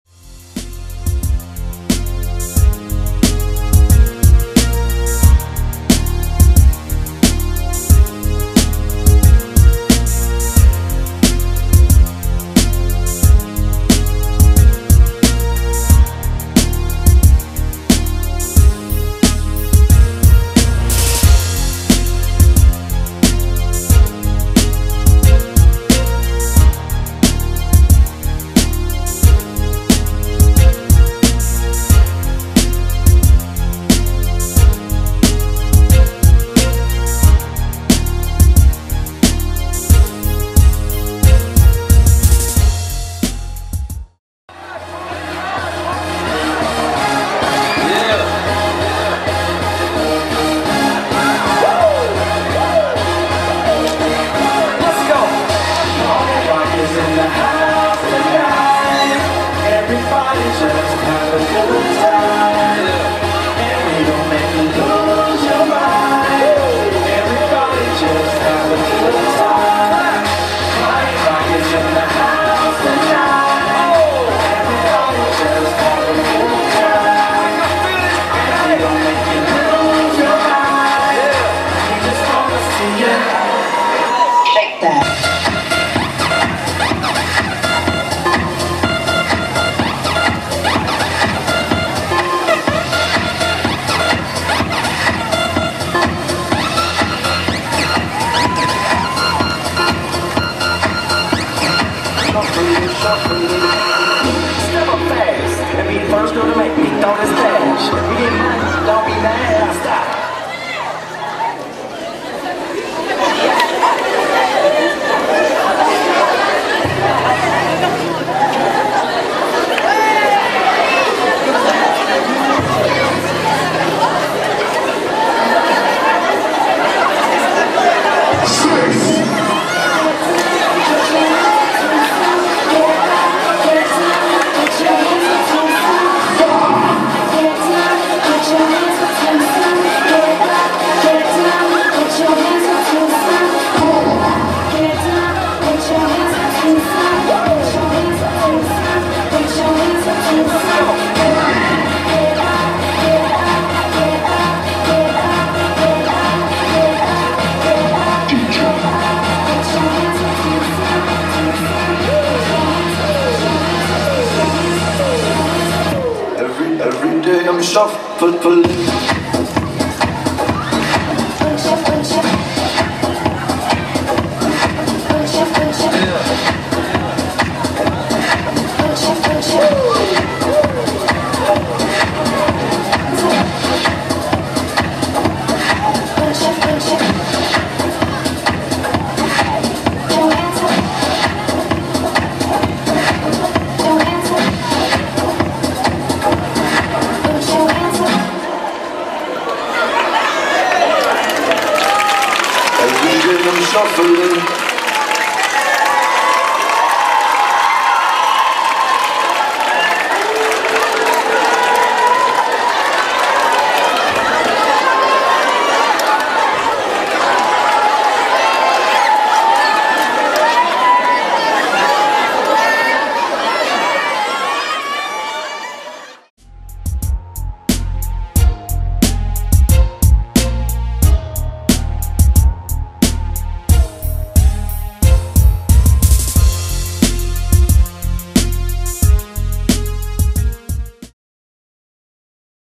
Flashmob Lucie Aubrac Givors 2012.mpg
Flashmob du Collège Lucie Aubrac à Givors, réalisé en Juin 2012.
Chorégraphie imaginée par Mr Gimenez, professeur du collège
Participation en nombre de tous ...
Réalisation Olivier Descours, MJC de Givors
A l' année prochaine pour une nouvelle choré !